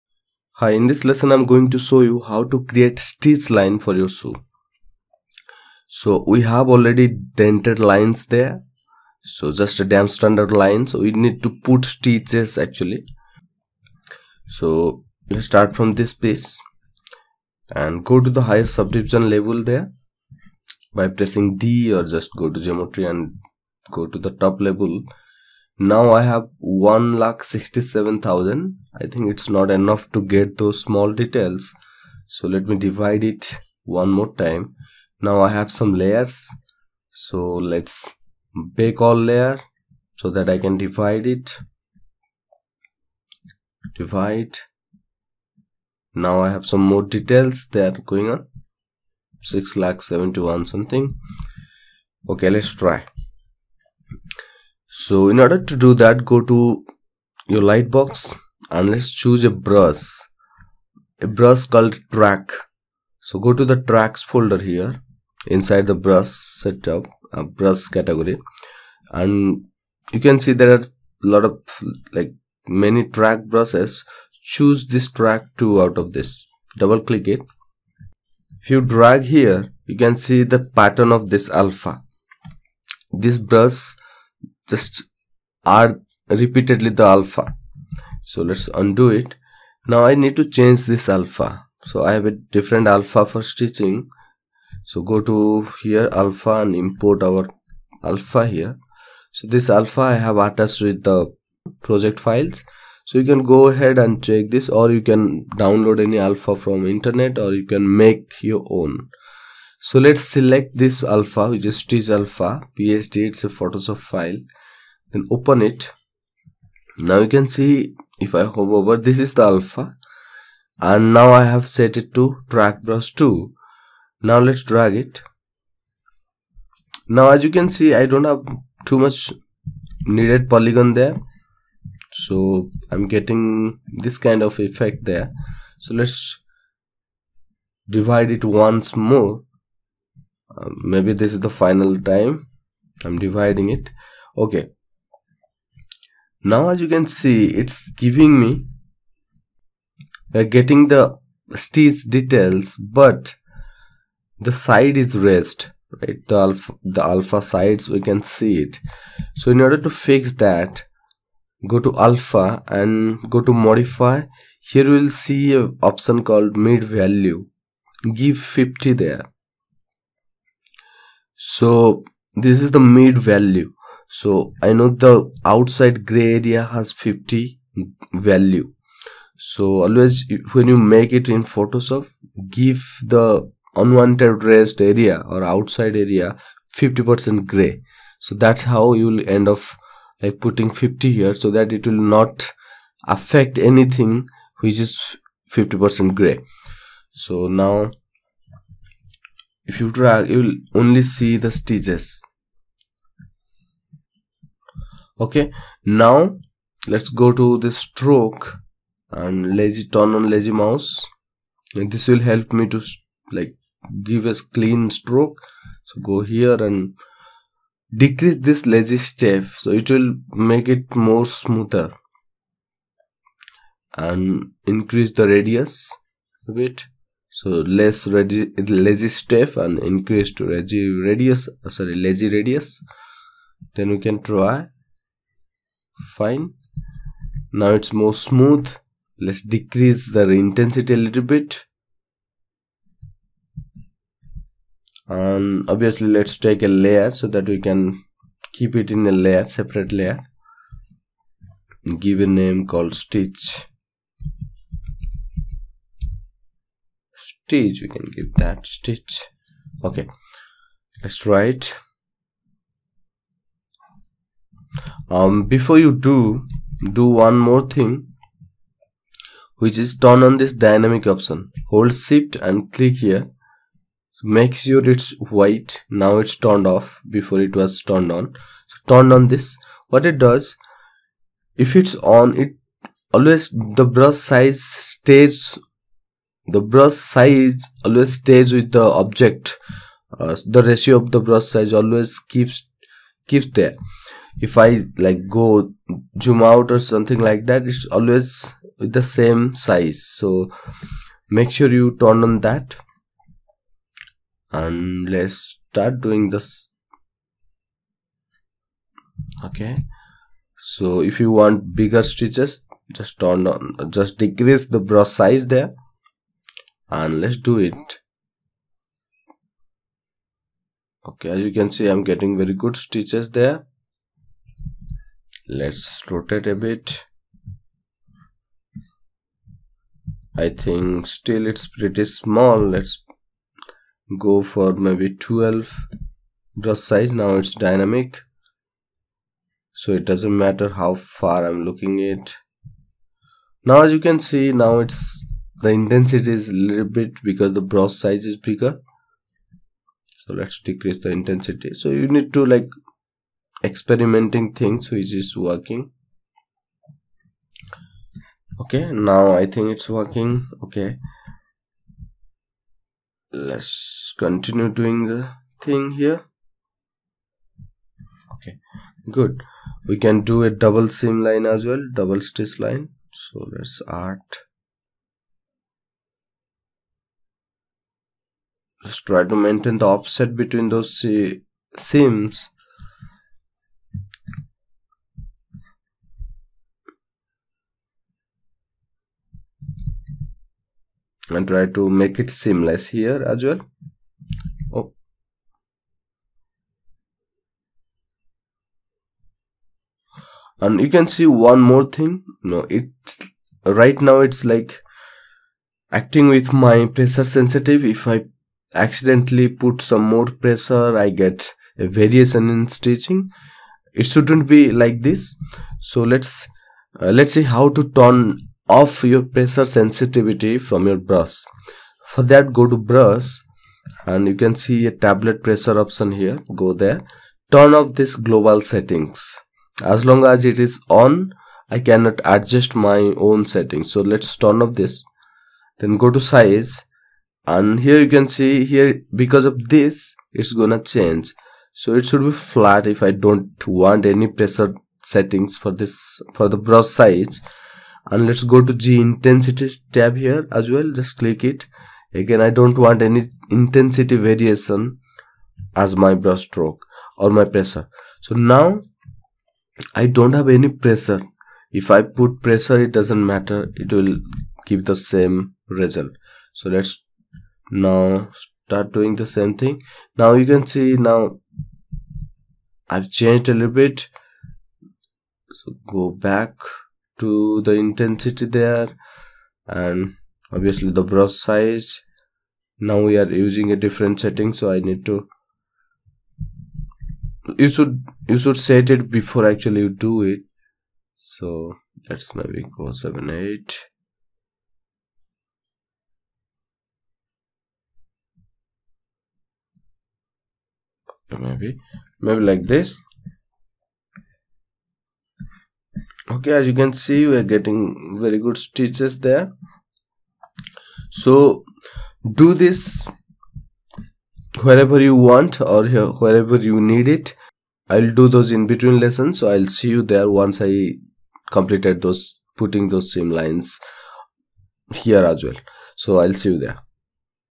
Zbrush tutorial : Adding Stich Details on shoes
Description : In this Zbrush tutorial : Adding Stich Details on shoes video I will show you how to do stitching detailing on any zbrush model.